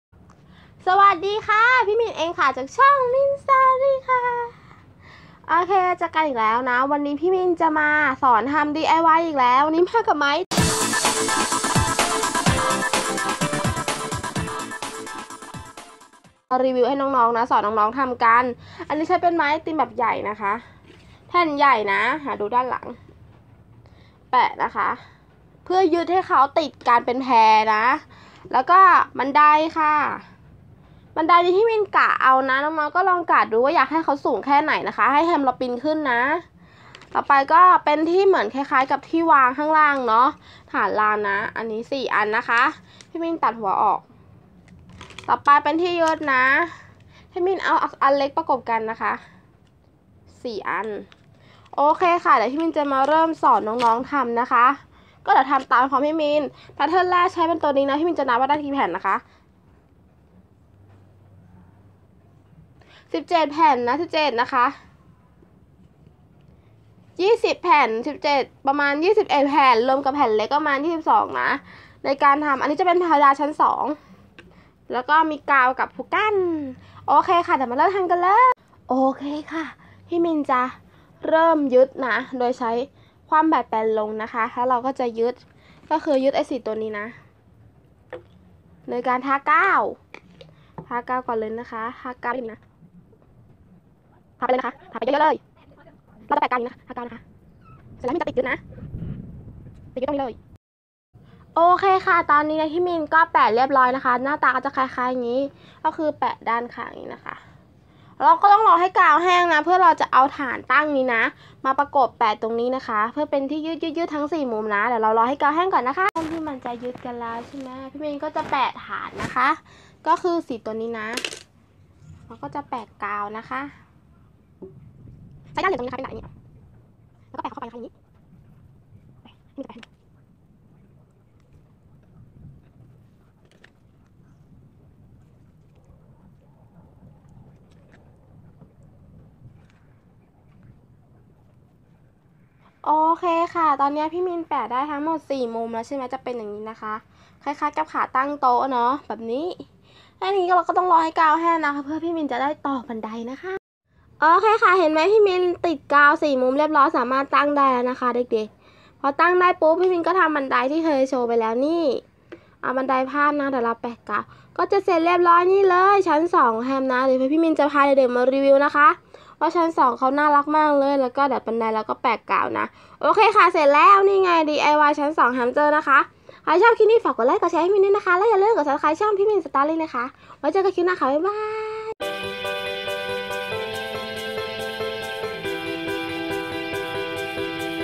พี่มีนสอนทำชั้นลอยแฮมสเตอร์ DIY จากไม้ไอติม Hamster / min story
❤❤ ติดตามพี่มีนและเด็กๆบ้านมี้มีนตามนี้เลยนะ ❤❤